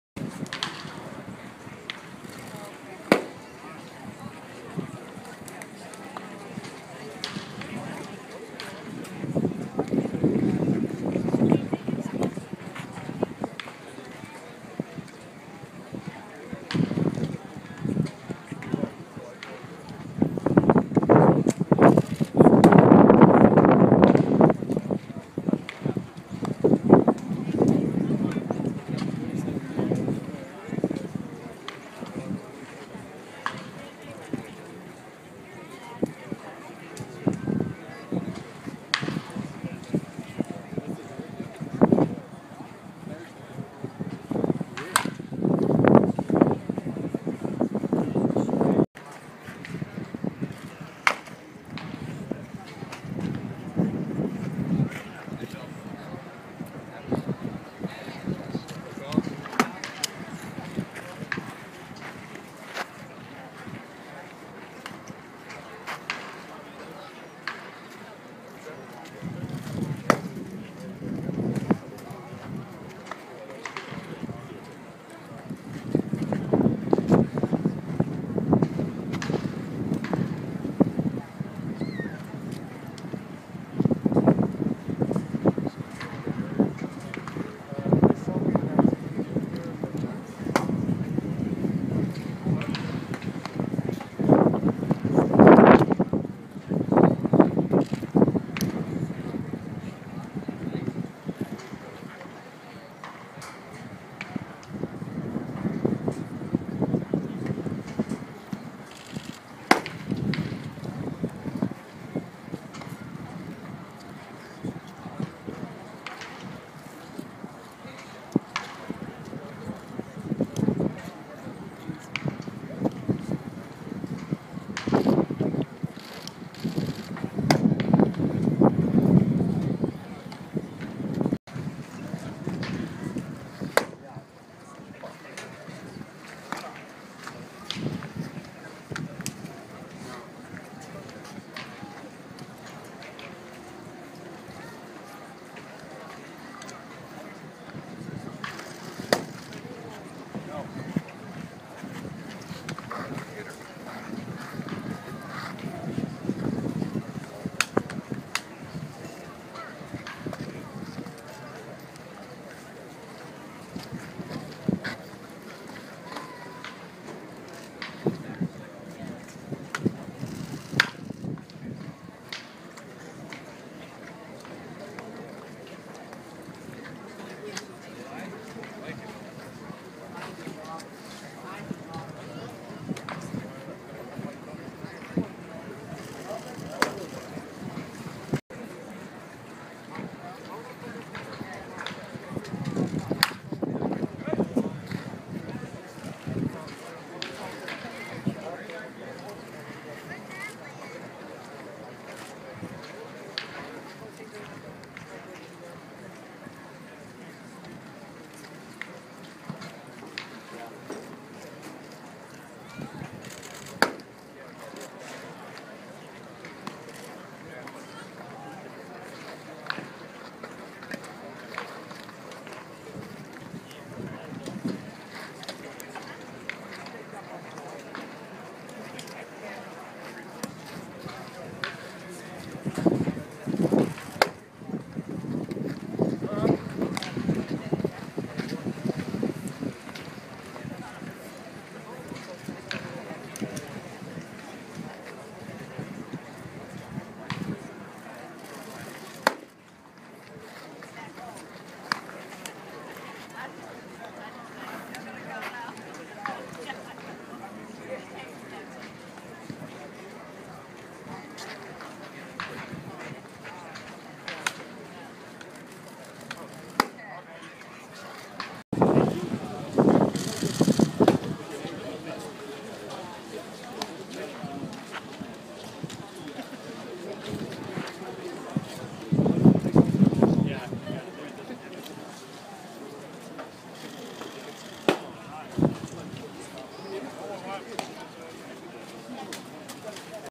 Henry Owens shows his BP stuff to Jonny Gomes, Will Middlebrooks
FORT MYERS, Fla.